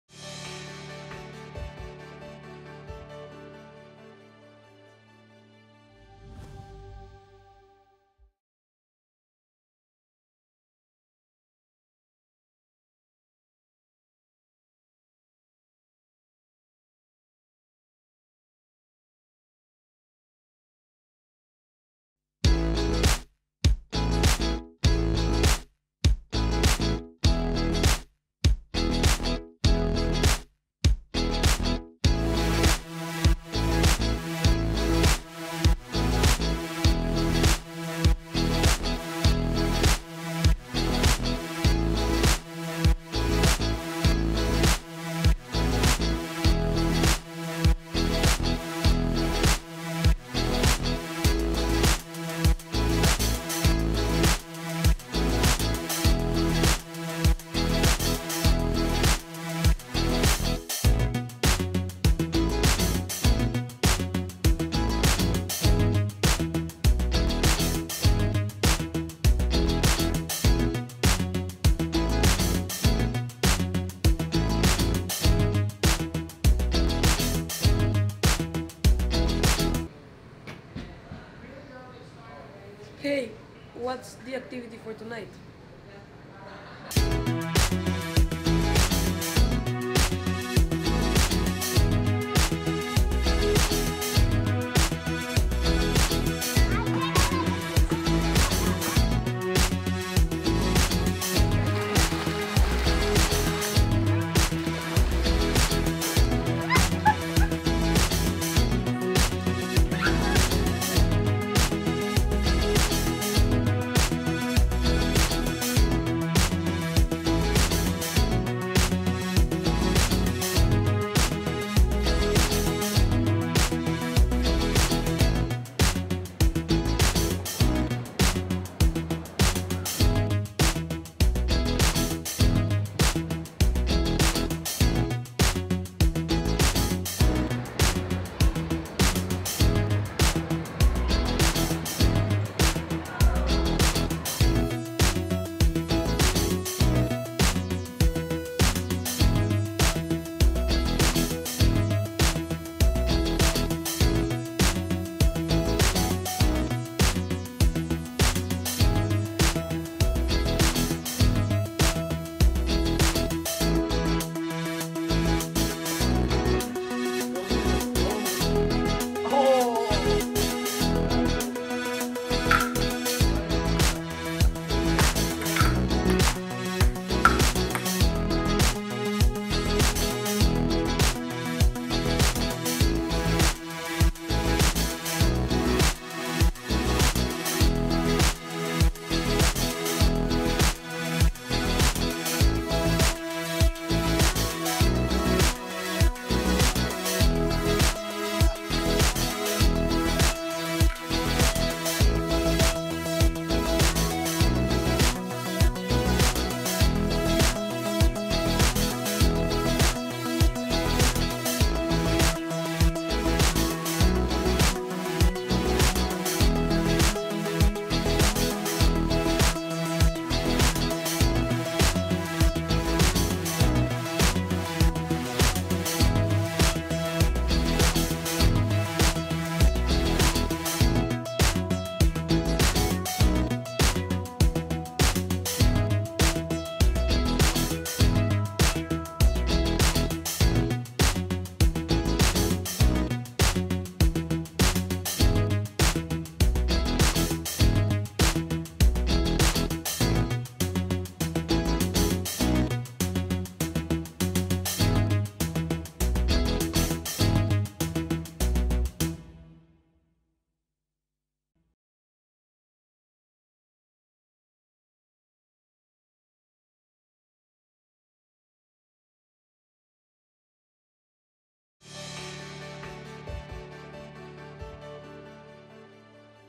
BGGA Summer Camp 2018 Memories
Take a look back at BGGA 2018 summer campers, learning & playing golf, taking part in fun activities and making new friends from around the world.